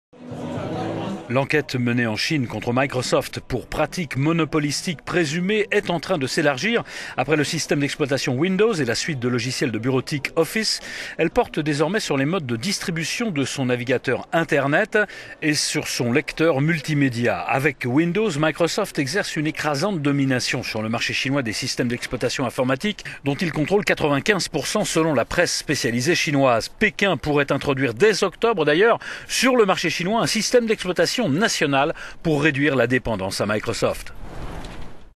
Microsoft : l'enquête s'élargit en Chine contre le géant informatique américain - economy
L'enquête menée en Chine contre Microsoft pour "pratiques monopolistiques présumées" est en train de s'élargir : après le système d'exploitation Windows et la suite de logiciels de bureautique Office, elle porte désormais sur les modes de distribution de son navigateur internet et de son lecteur multimédias.
Avec Windows, Microsoft exerce une écrasante domination sur le marché chinois des systèmes d'exploitation informatiques, dont il contrôle 95%, selon la presse chinoise.